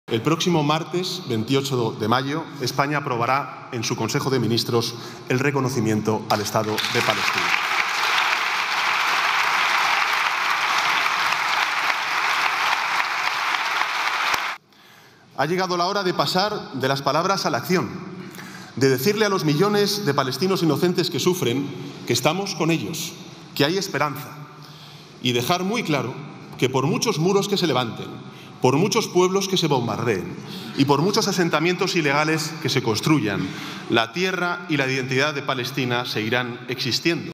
Sánchez anuncia que España reconocerá a Palestina como Estado el 28 de mayo
El presidente del Gobierno, Pedro Sánchez, ha anunciado este miércoles que el Consejo de Ministros aprobará el reconocimiento de Palestina como Estado por España el próximo 28 de mayo. El jefe del Ejecutivo ha explicado que la decisión ha sido consensuada entre los dos socios del Gobierno. Y haciéndose eco del sentir mayoritario de los ciudadanos españoles.